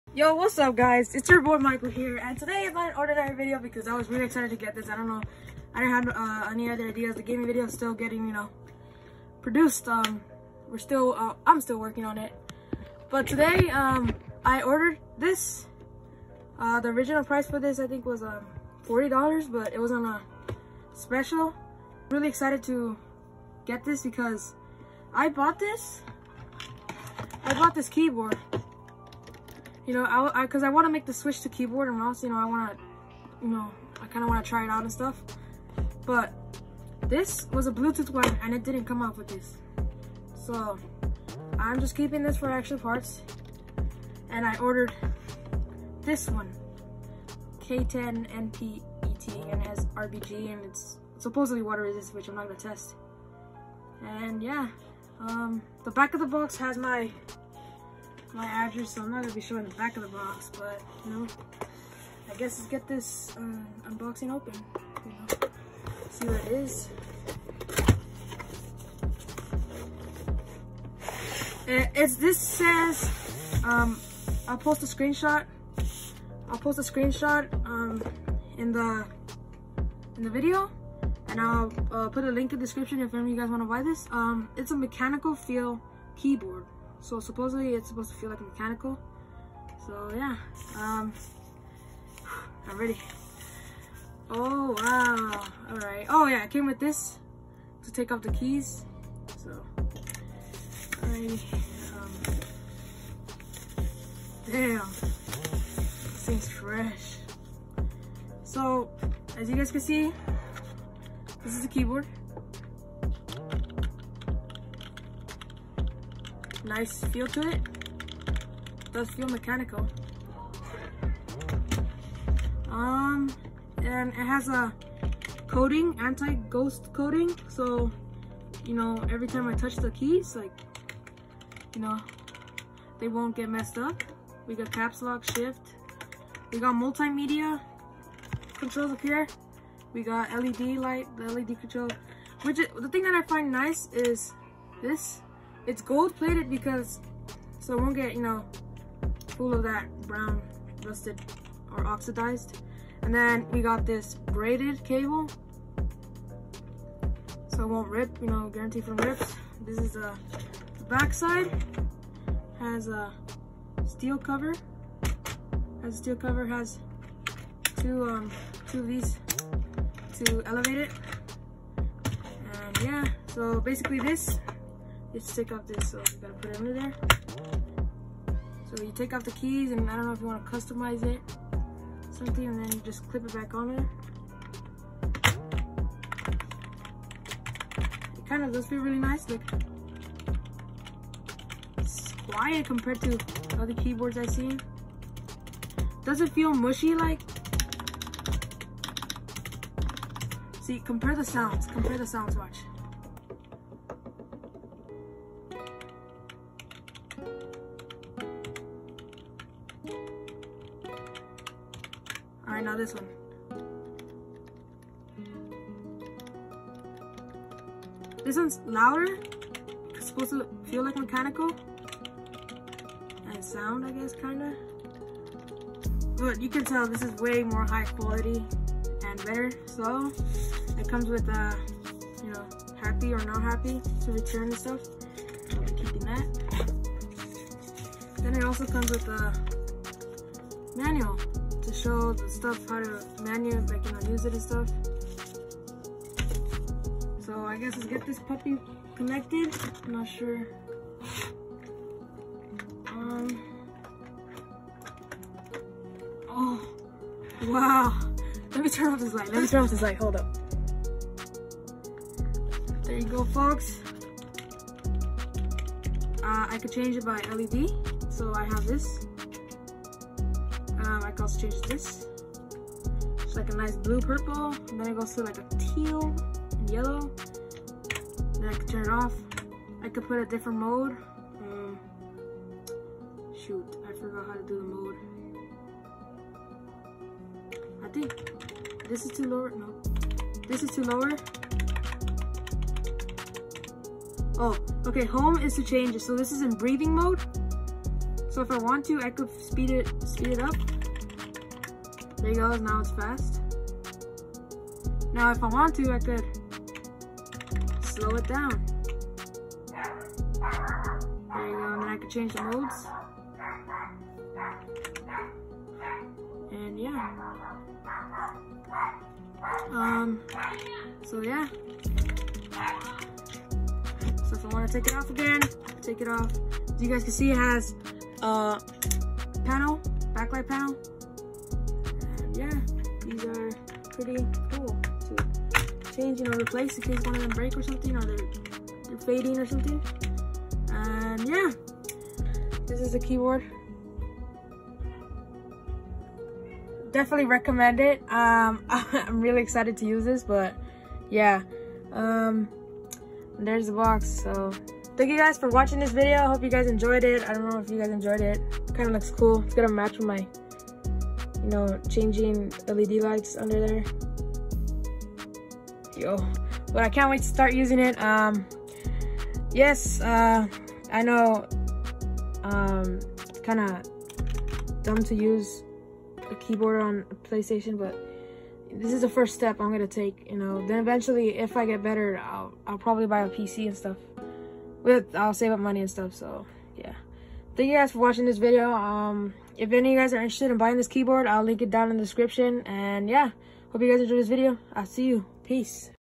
Npk10 keyboard unboxing!!
NPET K10 Gaming Keyboard USB Wired Floating Keyboard, Quiet Ergonomic Water-Resistant Mechanical Feeling Keyboard, Ultra-Slim Rainbow LED Backlit Keyboard for Desktop, Computer, PC

Turn on post notifications for alerts for new video’s!